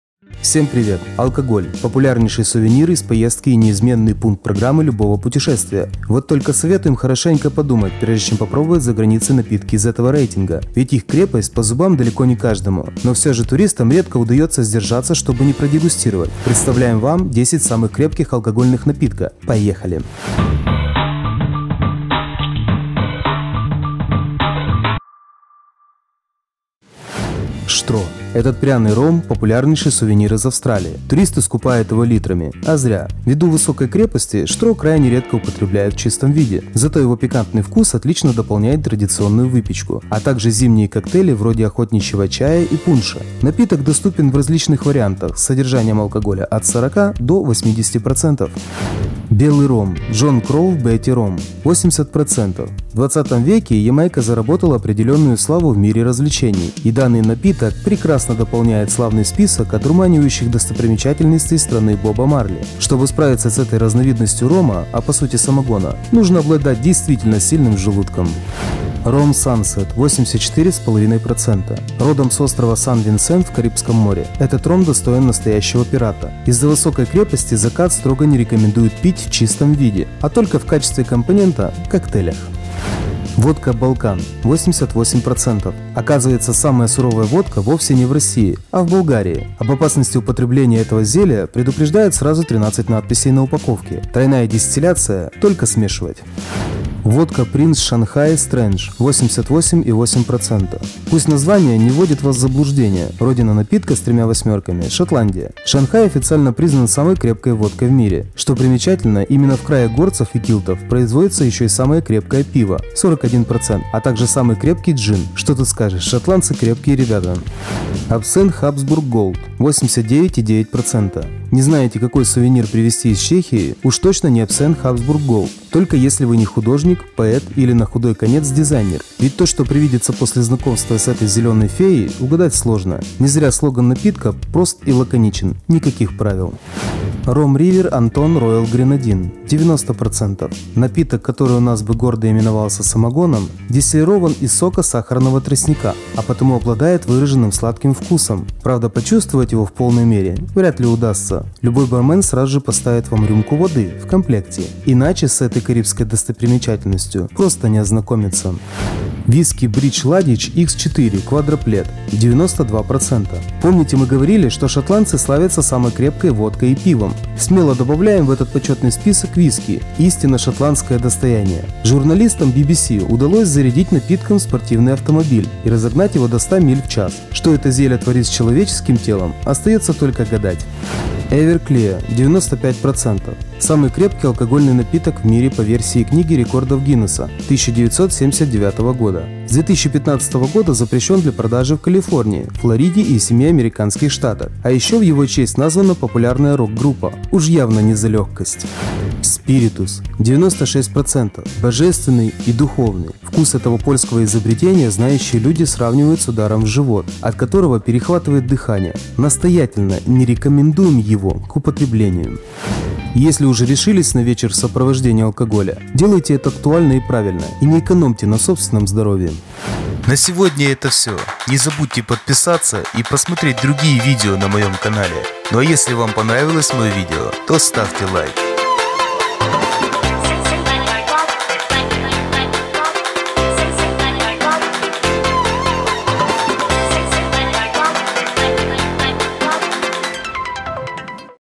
10 самых крепких алкогольных напитков.
Всем привет , Алкоголь – популярнейший сувенир из поездок и неизменный пункт программы любого путешествия. Вот только советуем хорошенечко подумать, прежде чем пробовать заграницей напитки из этого рейтинга. Ведь их крепость по зубам далеко не каждому. Но, всё же, туристам редко удаётся сдержаться, что бы не продегустировать… представляем вам 10 самых крепких алкогольных напитка.

На развитие и поддержку канала, можно задонатить на эти кошельки:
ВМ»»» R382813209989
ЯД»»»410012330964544